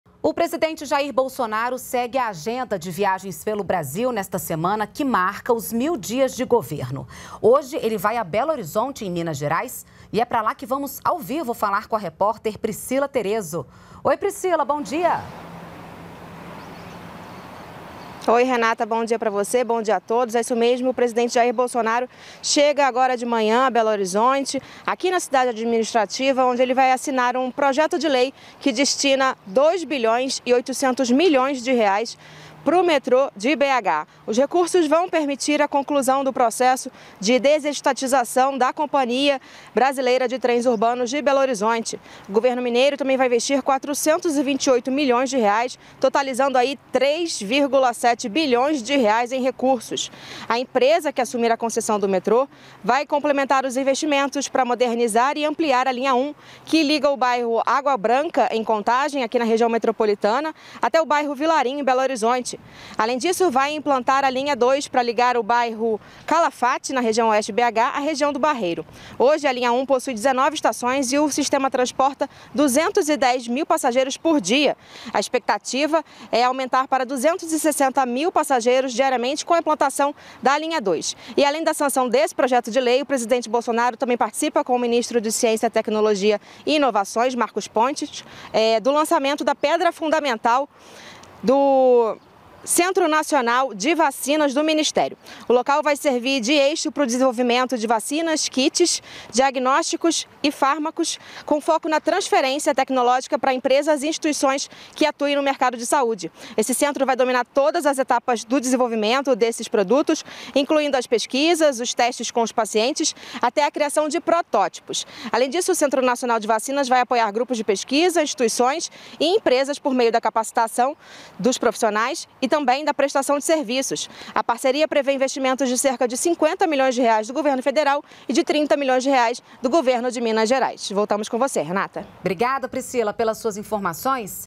Agenda do presidente em Belo Horizonte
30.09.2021 – Em Belo Horizonte, Minas Gerais, o presidente Jair Bolsonaro anuncia recursos para o metrô e a construção do Centro Nacional de Vacinas.